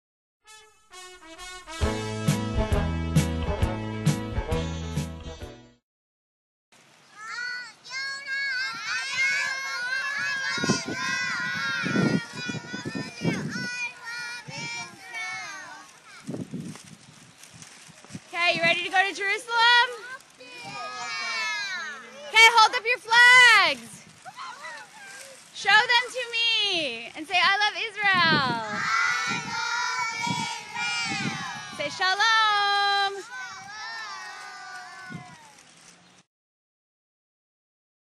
Austin Yom Hatzmaut.wmv
The Dolphin preschool  class from Austin sends this video with love to all of the children of Israel